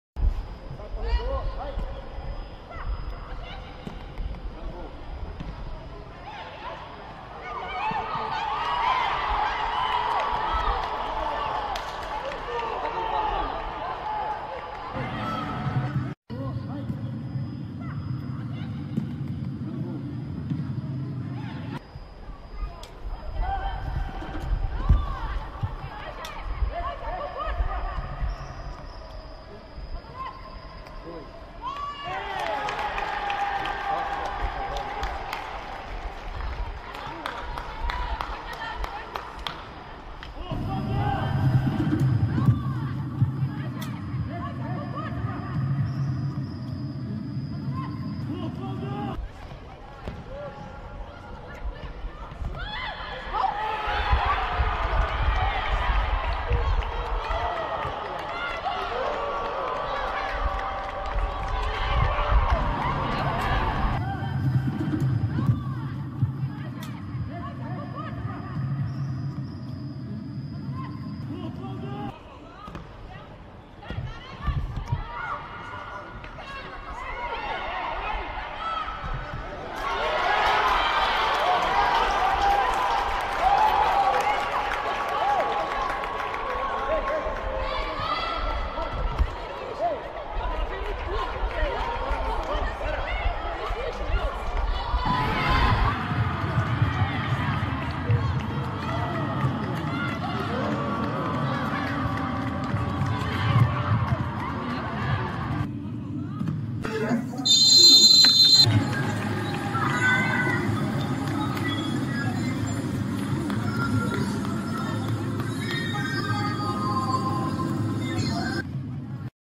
Gol do Flamengo x Iranduba Brasileiro Feminino
Flamengo campeonato brasileiro Feminino teve golaço.